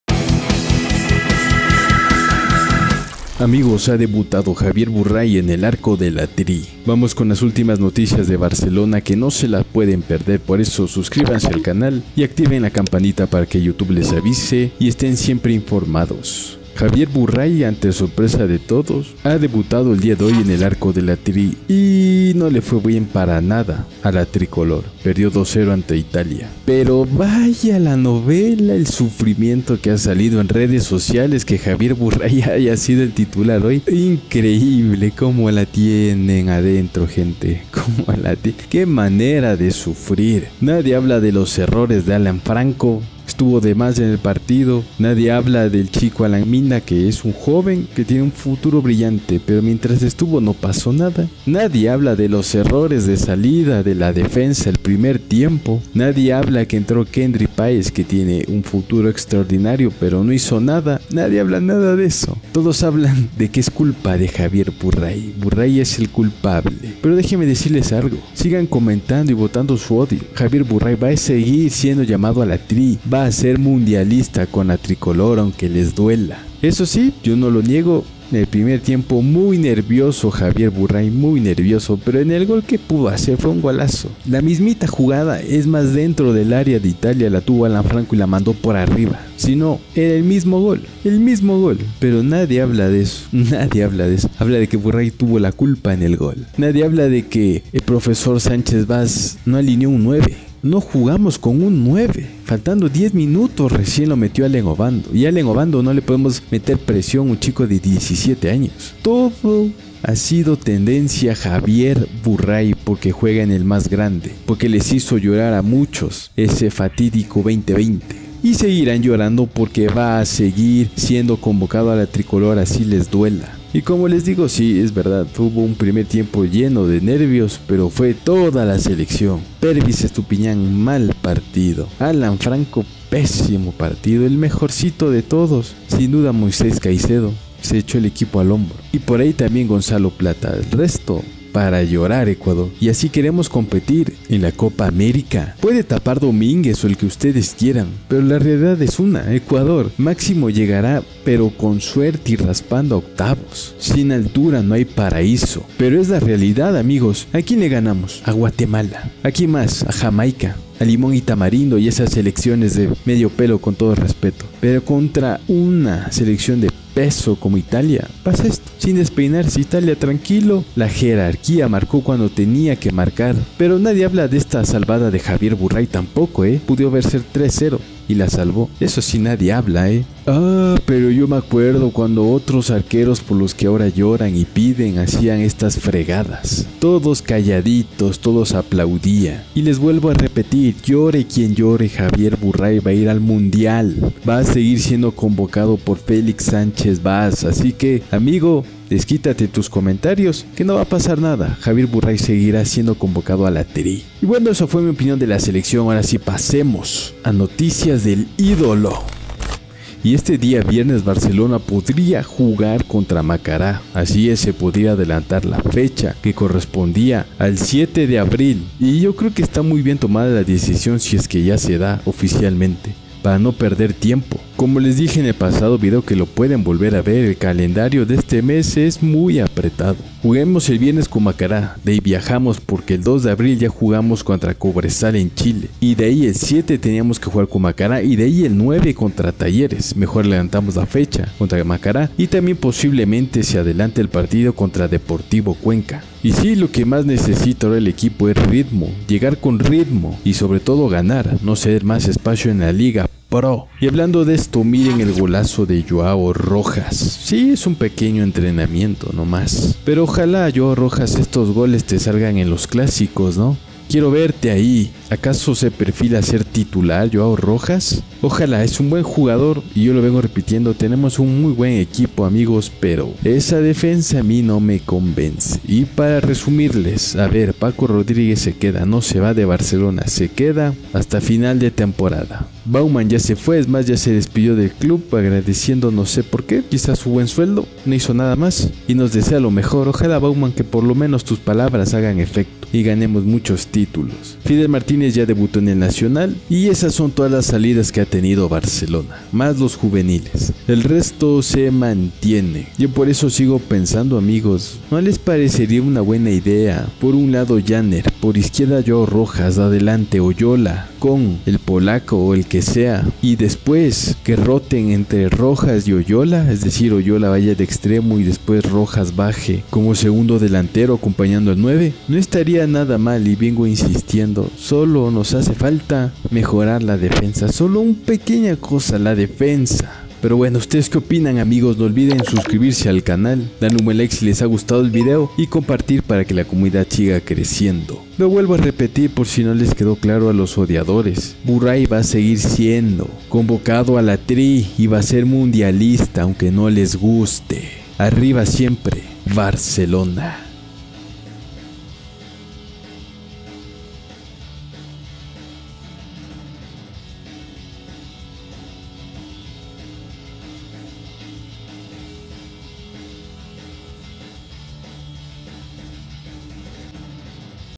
DEBUT DE JAVIER BURRAI Emociona a TODO el ECUADOR / Cambio de PLANES en BARCELONA SC
Noticias de ultimo minuto Barcelona SC , Javier burrai debuto tapando con la tri frente a Italia, hay cambio de planes en Barcelona por la liga pro

✅No olviden activar las notificaciones 🔔😁
! Un abrazo ¡